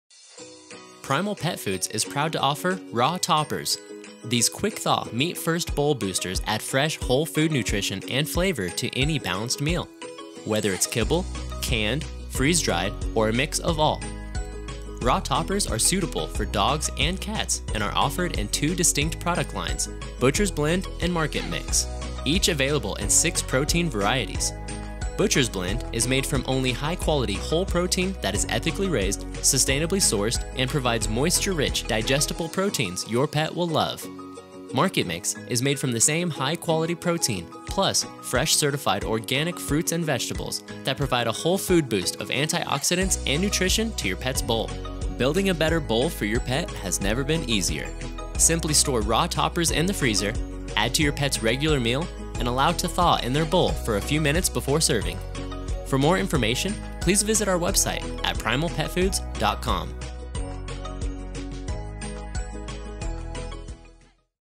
Raw Toppers | Primal Pet Foods
Quick-thaw, raw meat toppers for dogs and cats made from high quality protein and organic produce.